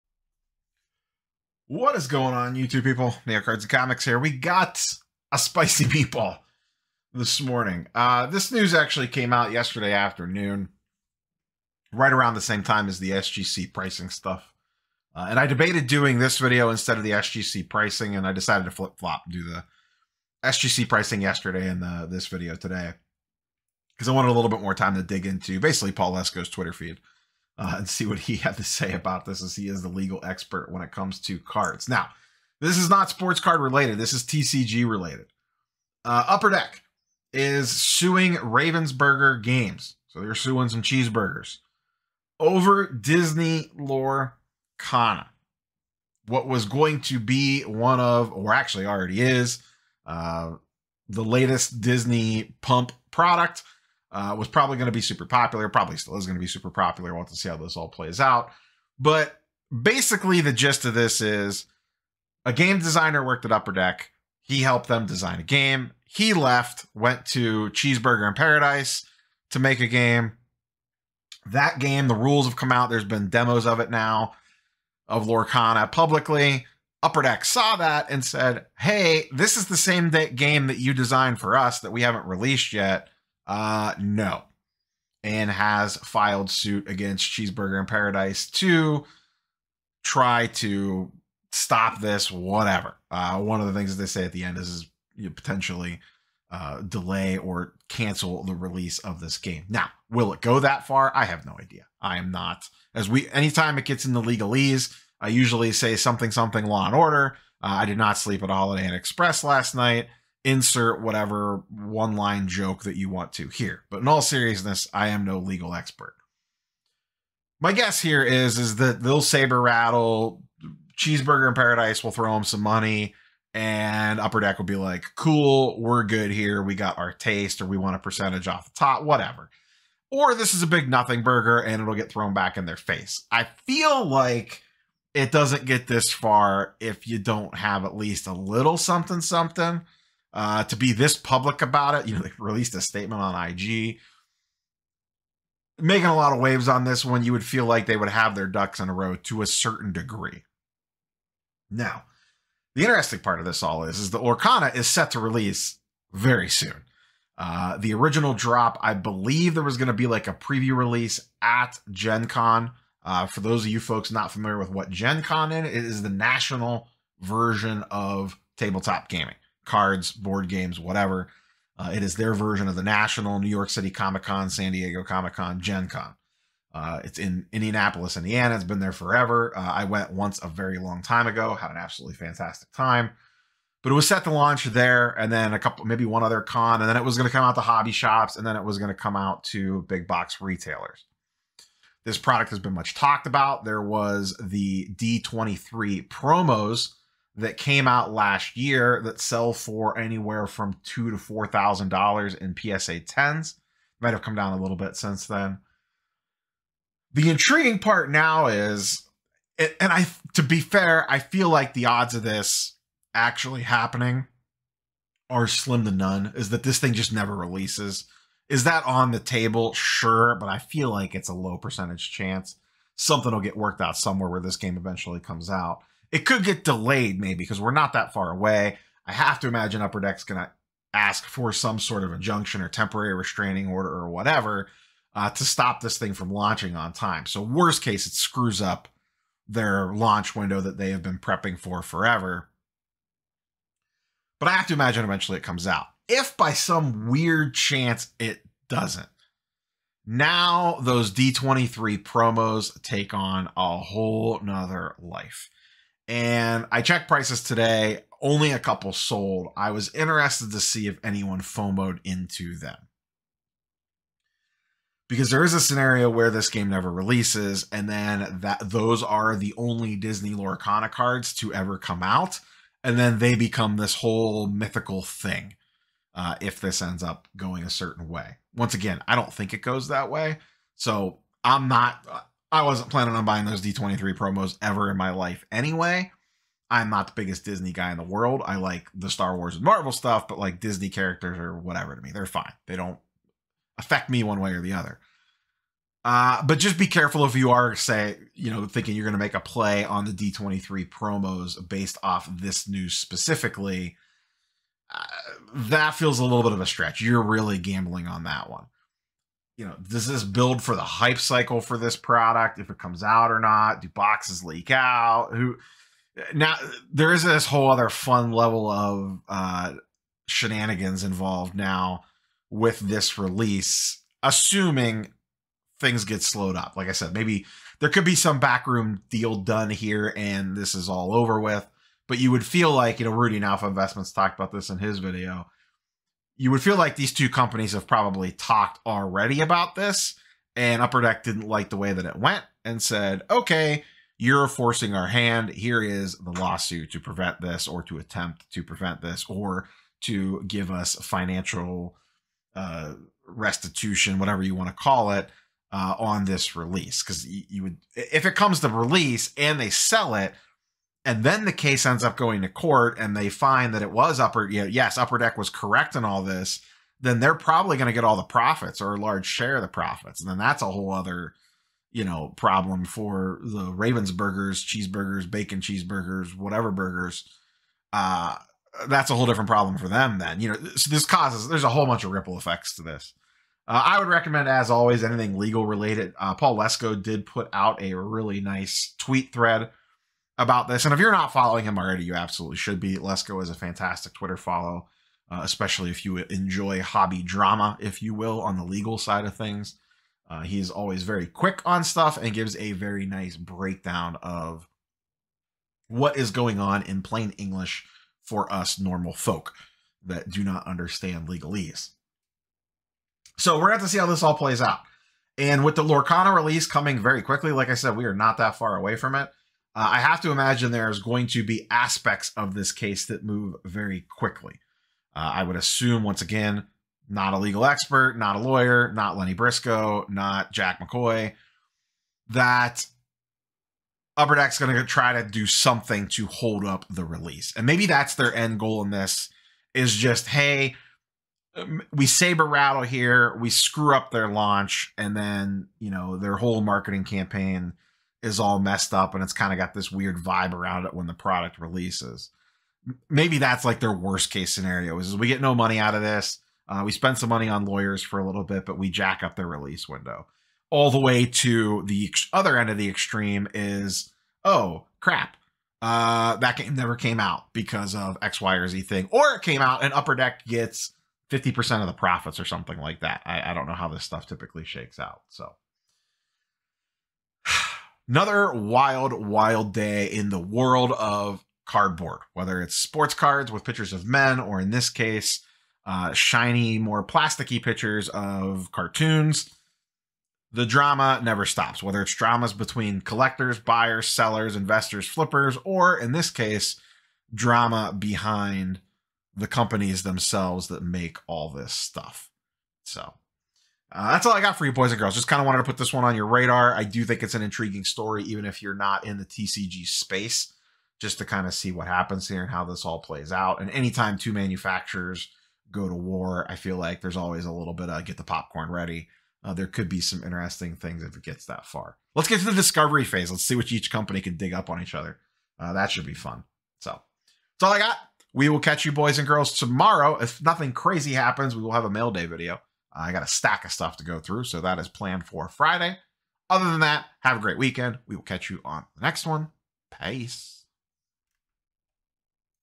Upper Deck sues Ravensburger Games over the release of Disney Lorcana.
Upper Deck is is going after Disney Lorcana and potently trying to stop or slow down the release of the product.

"As an Underdog Fantasy Associate I earn from qualifying deposits via my affilate link"
“As an eBay Associate I earn from qualifying purchases.”
“As an Fanatics Associate I earn from qualifying purchases.”